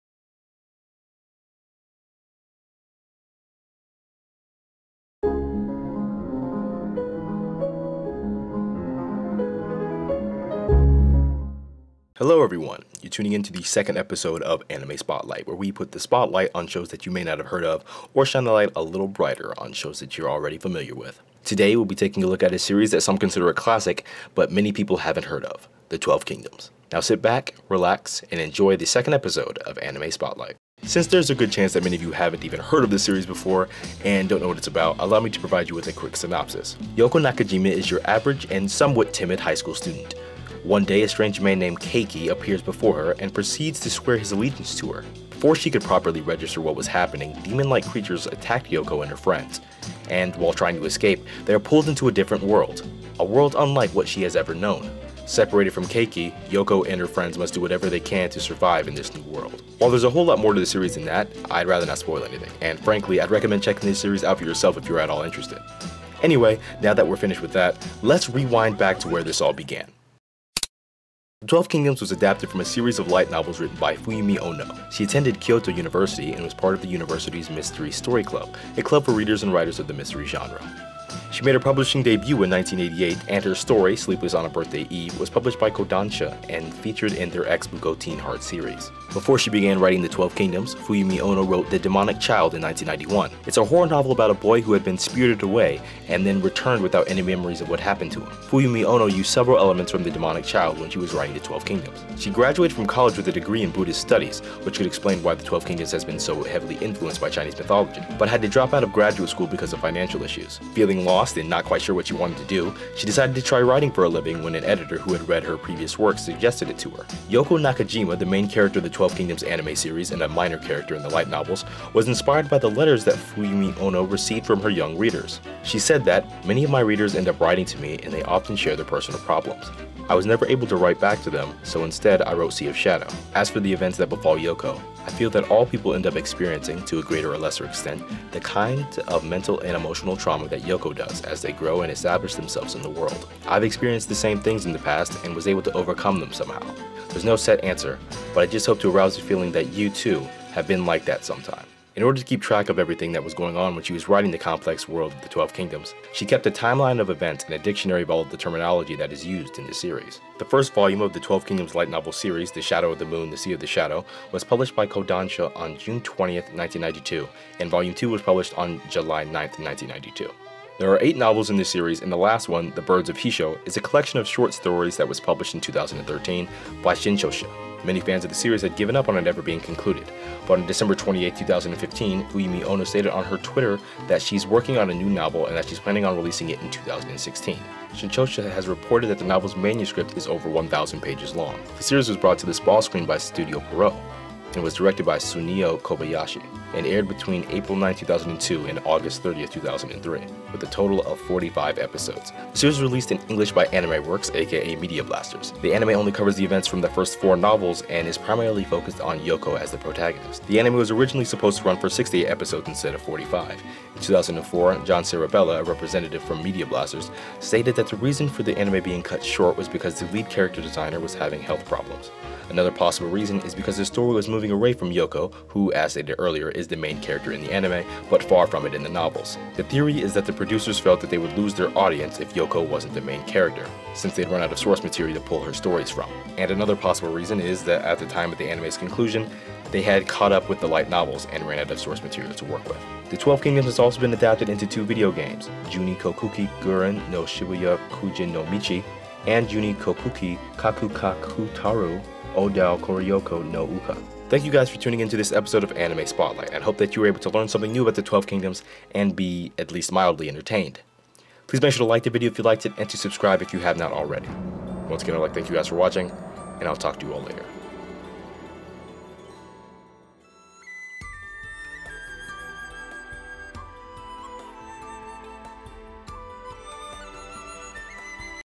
A Closer Look at The Twelve Kingdoms - Anime Spotlight
Hello, everyone, you’re tuning in to the second episode of Anime Spotlight where we put the spotlight on shows that you may not have heard of, or shine the light a little brighter on shows that you’re already familiar with. Today, we’ll be taking a look at a series that some consider a classic, but many people haven’t heard of, The Twelve Kingdoms (Juuni Kokuki).